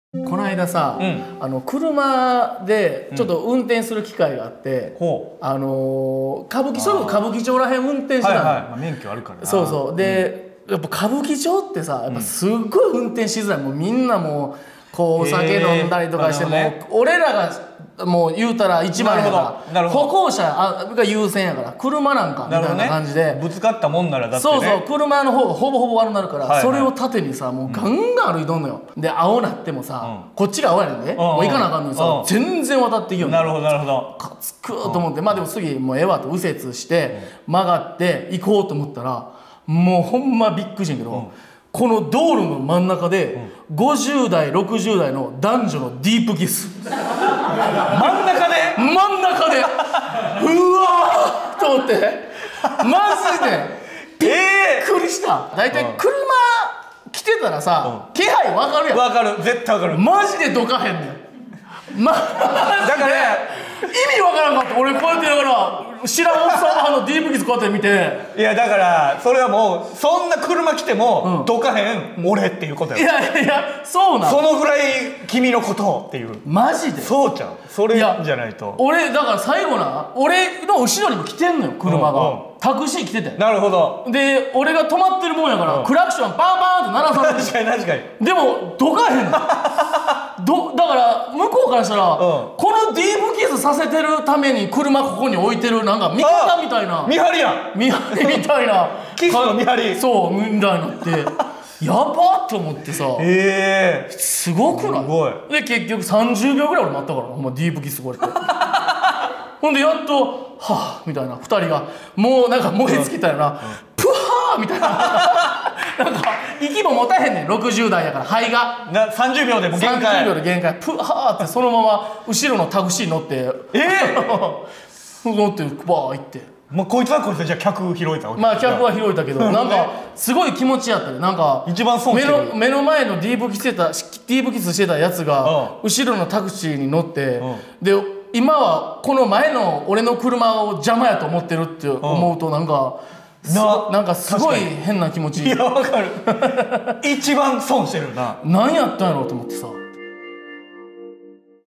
歌舞伎町の道路の真ん中でカップルが!? よしログ
2017年04月20日（木）田畑藤本のよしログ。久しぶりに車を運転する機会があったという田畑藤本の田畑。人混みで運転しづらい新宿の歌舞伎町を通ることになったのだが、ラブホテル街の角を曲がったところでとあるカップルの信じられない光景を目撃し……。周囲がホテルなだけに、道端での行為に田畑も疑問を感じたという。